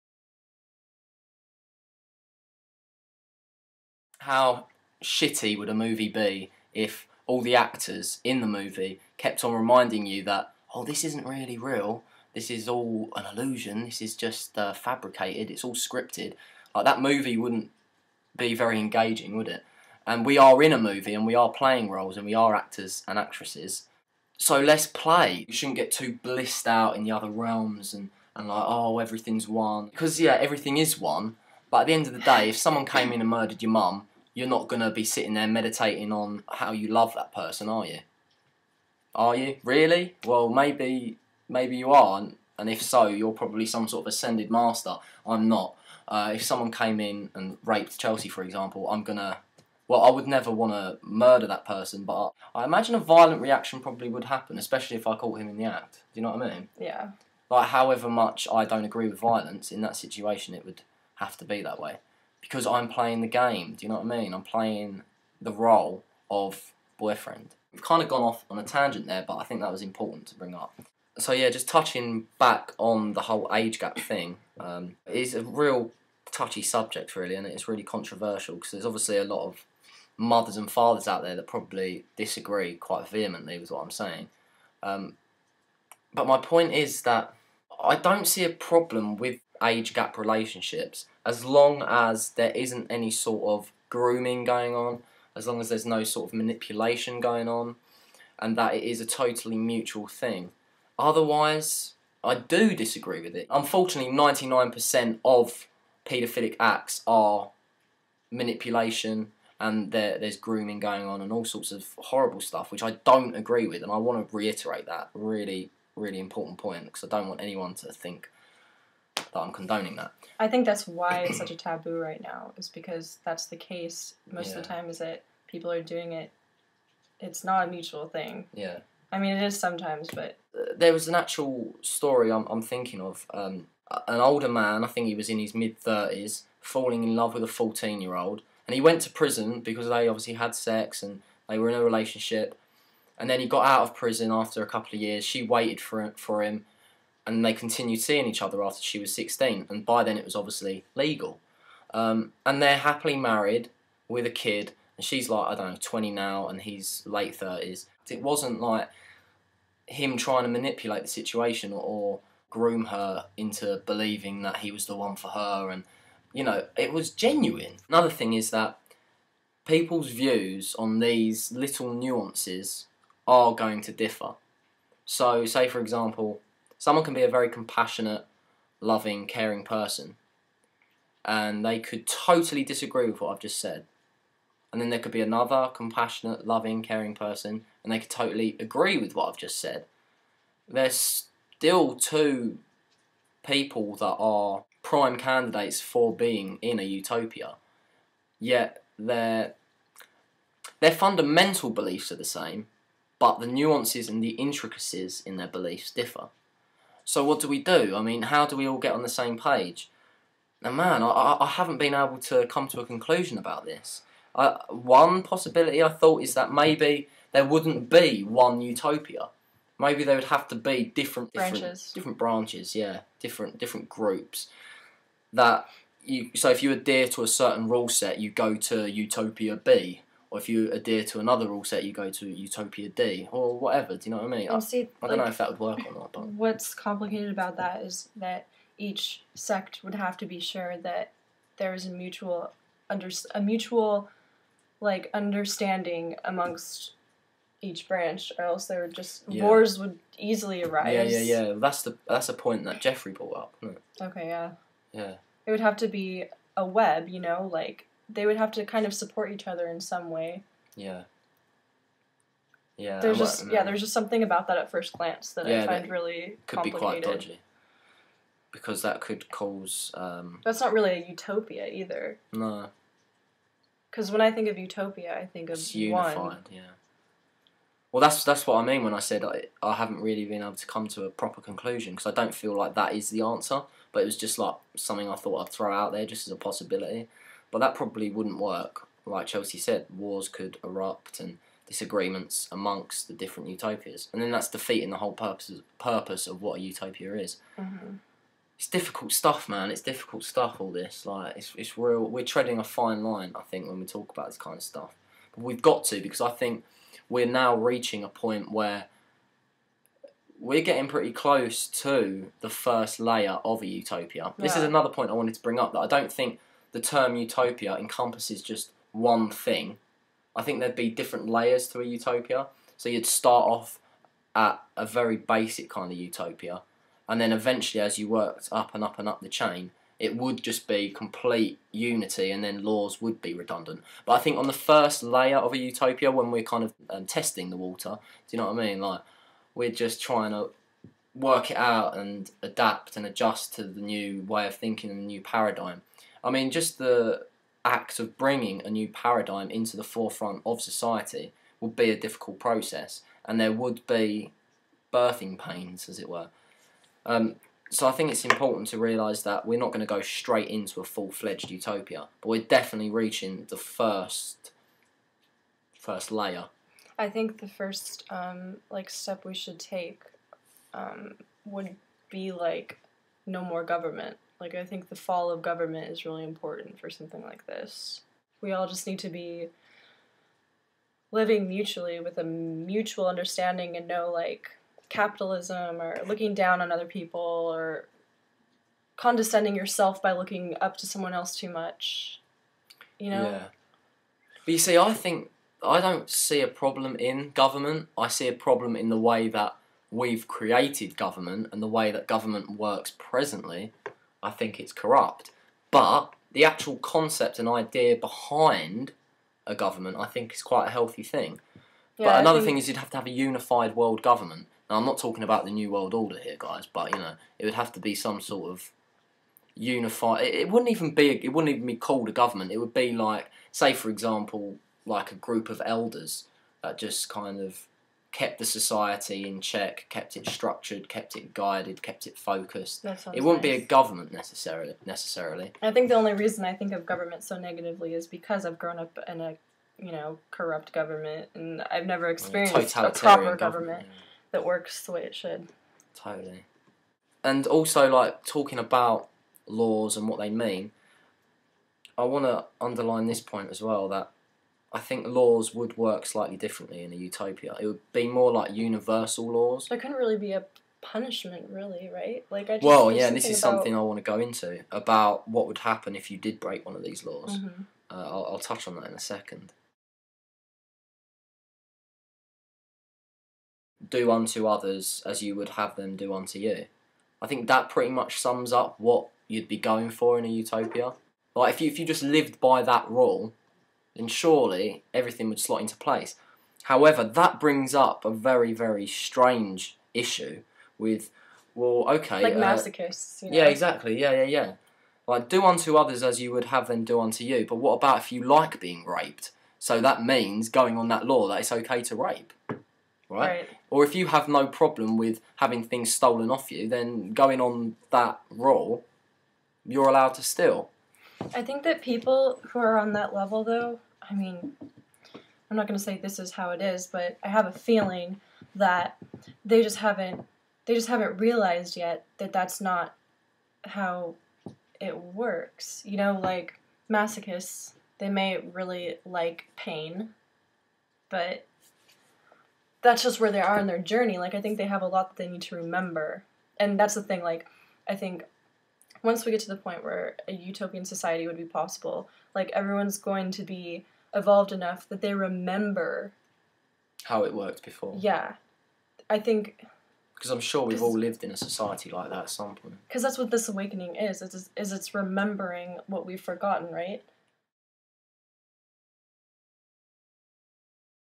Reece & Chelsea (S1-P2)
Reece & Chelsea discussing how law would work in a Utopian society.